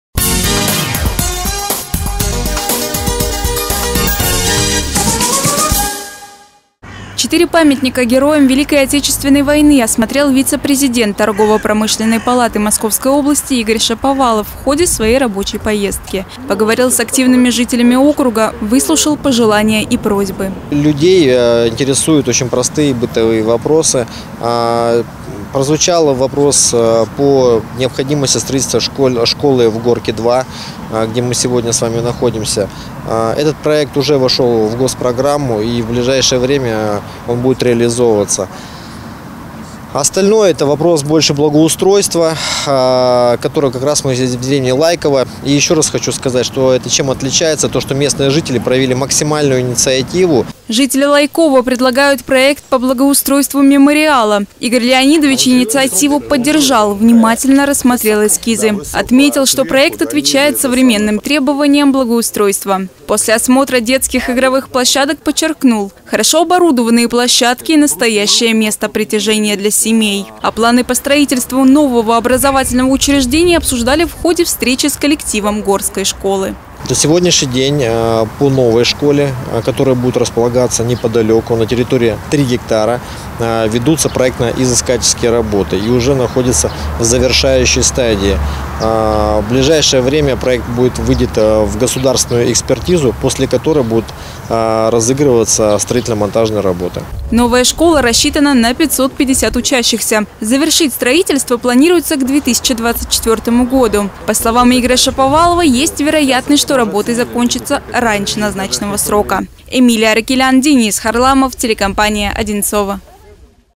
Игорь Шаповалов побывал в ТУ Горское
Вице-президент Торгово-промышленной палаты Московской области Игорь Шаповалов побывал с рабочим визитом в теруправлении Горское. Встретился с жителями и обсудил самые острые проблемы.